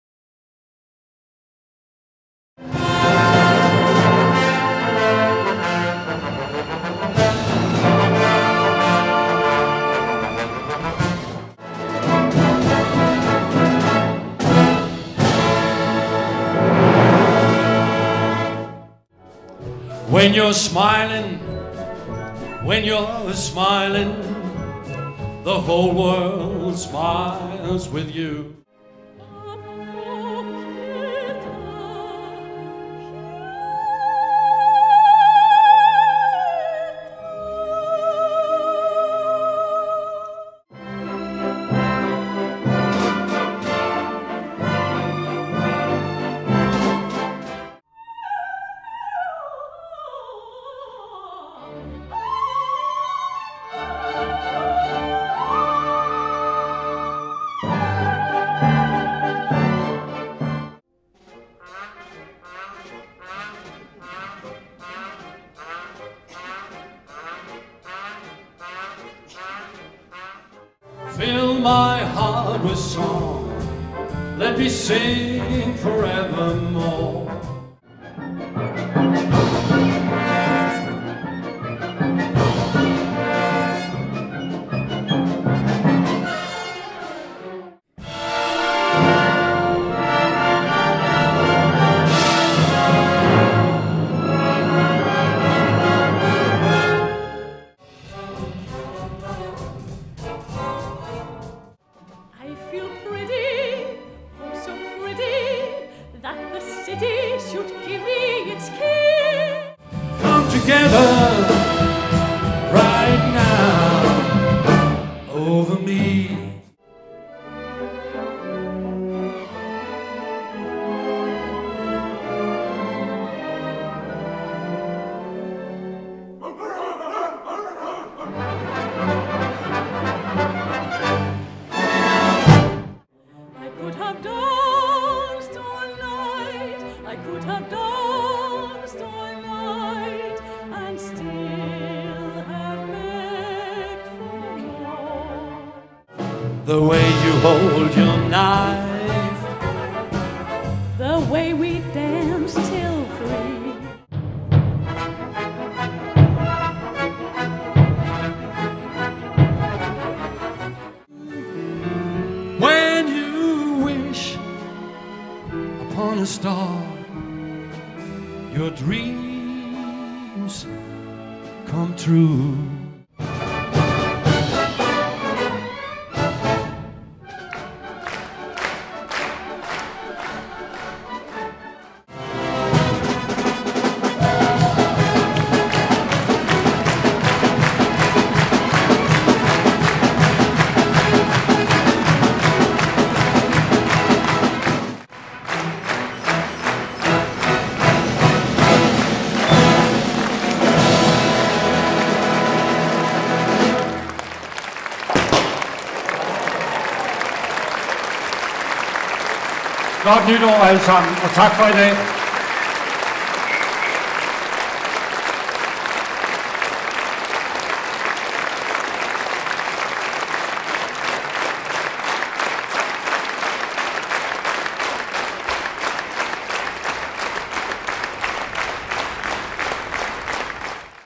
Highligts fra Nytårskoncert 2019 med Charlotte Hjørringgaard Larsen og Søren Pilmark
Orkester MidtVest
Dirigent: Ole Faurschou
Holstebro Musikteater, januar 2019